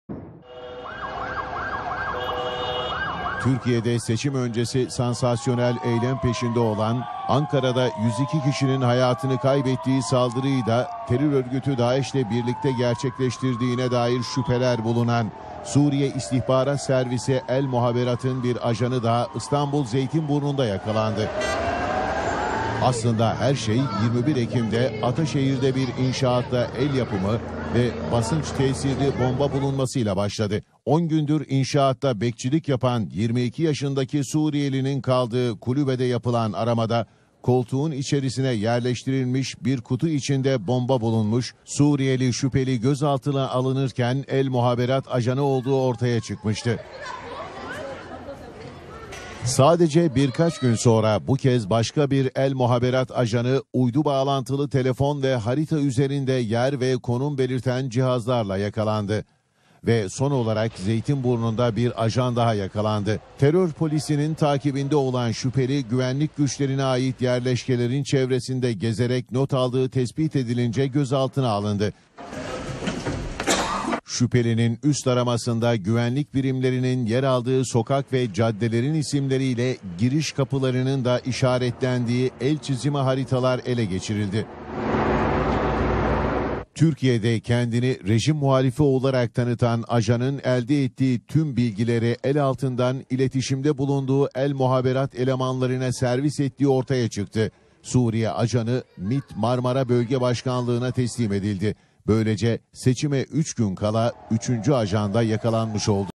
9 günde 3 ajan yakalandı
Türkiye'de seçim öncesi 9 günde 3 Sriye ajanı yakalandı...